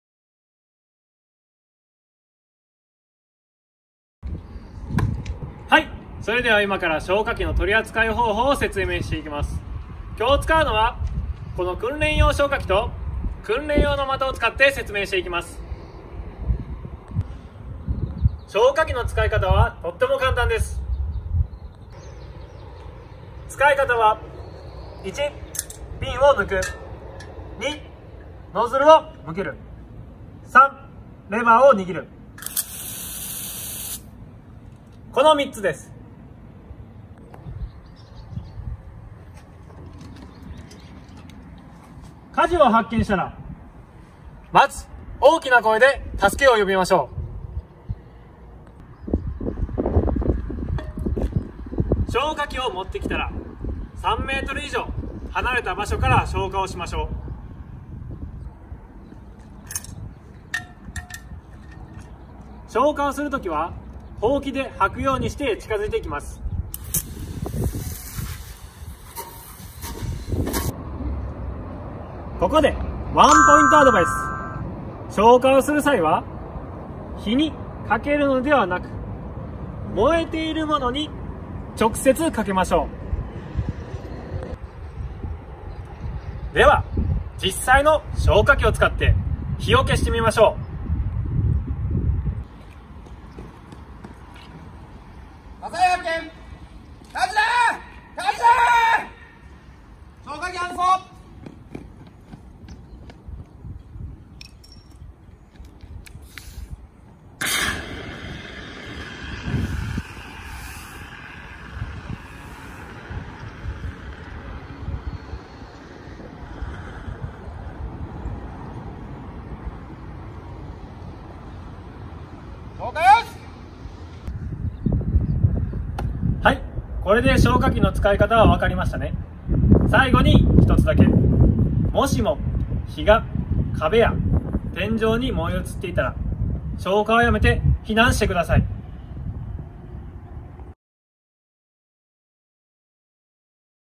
【高島市消防本部】 消火器の正しい使い方
「あなたも消火器を正しく使える人に！」
この動画では、現役の消防士が消火器の使い方について分かりやすくお伝えします。

■消火器の使い方のポイント■

○ピンを抜く・ノズルを向ける・レバーを握る

○消火は燃えている物に直接かける

○火が天井に燃え移ったらすぐに避難

高島市消防本部では防災意識向上のきっかけになるようにこれからも消防、防災に関する動画を発信していきます。

◆高島市ホームページ

◆高島市消防本部ホームページ